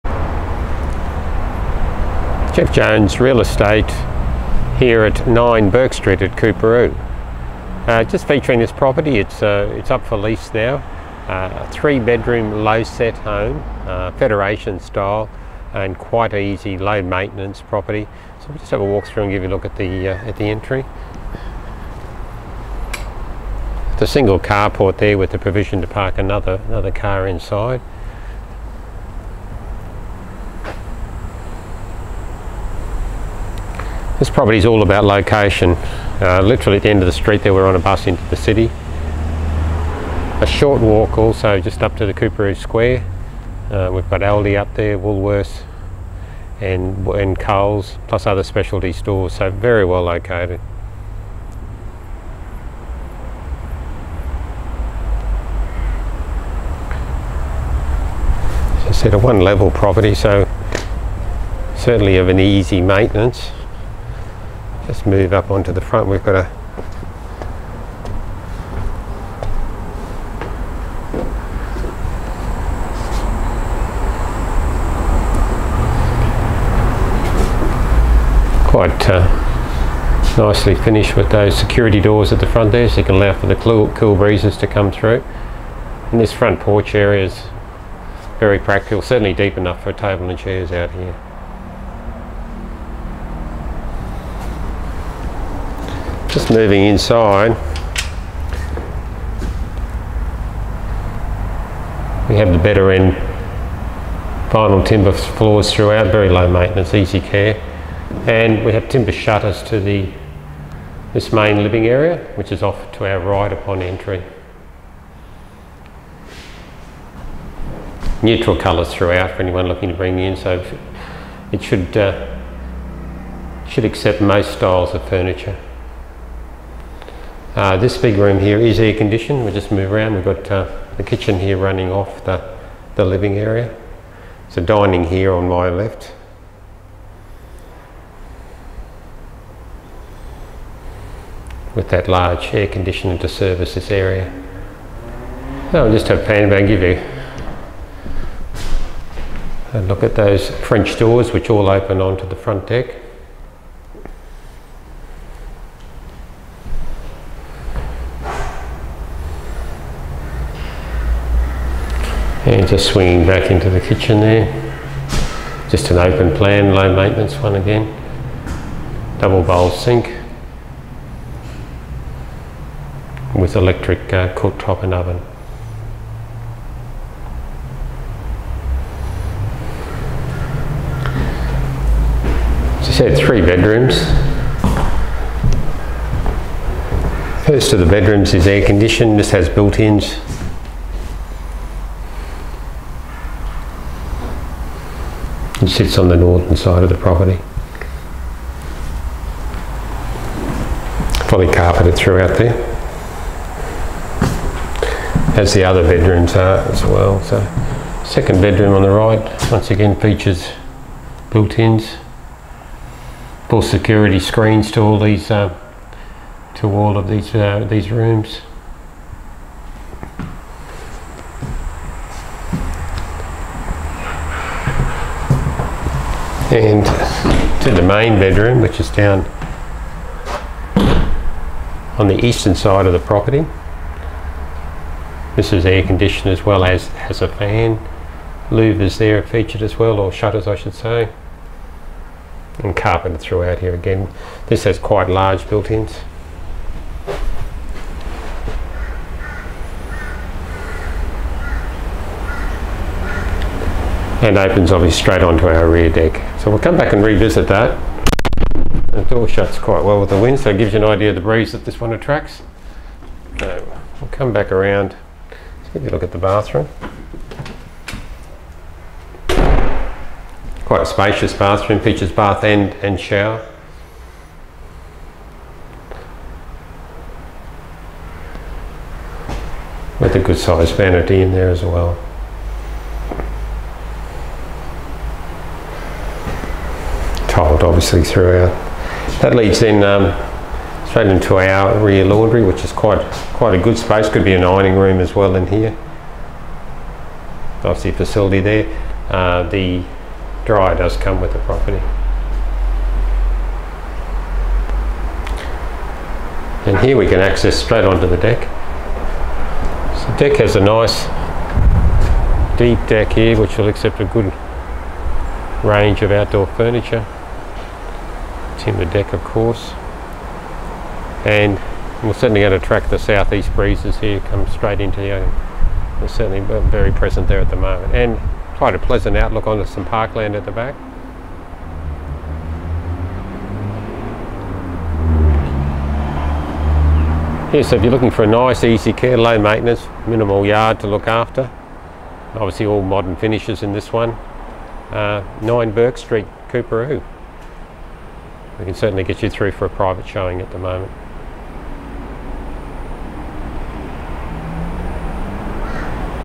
Let's Take A Walk Through 9 Burke Street, Coorparoo
This spacious brick home offers quality family living, right in the heart of Coorparoo. You'd be hard-pressed to find a better position than this location, which is surrounded by parklands, sporting fields, close to Coorparoo train station as well as affluent private schools and Coorparoo Secondary College.

At the front of the property, you'll find a large, air-conditioned living area with a modern and practical kitchen, opening to the front private patio - perfect for your morning coffee.
Down the hall, there are three bedrooms all decked out with built-ins, and air conditioning and a family-sized bathroom with separate toilet.

The extras keep coming with a large laundry, leading out to the massive rear deck overlooking endless parks.

- Double carport
- Air conditioning
- Modern flooring and carpet in bedrooms
- Separate shower and bath
- Plantations shutters
- Excellent security
Fully fenced yard

Close to Old Cleveland Road but away from the hustle and bustle, this perfect location is close to shopping, transport and several schools.